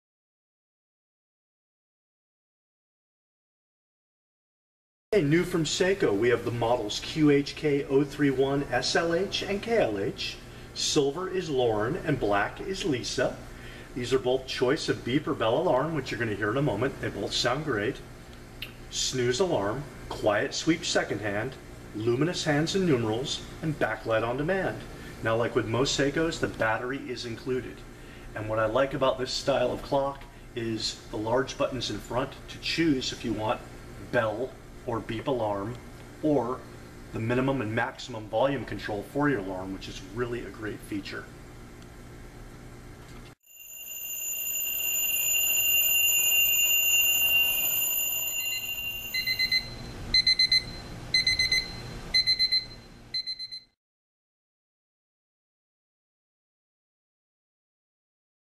Seiko QHK031KLH & QHK031SLH Metal Case Bedside Alarm Clock
The New Seiko Battery Operated Alarm Clock features a beautiful silver tone metal case. Choose between a bell or beep alarm. The quiet sweep second hand won't keep you awake. Luminous hands and numerals allow you to see the time in a dark room, and if you want more visibility there is an on demand backlight.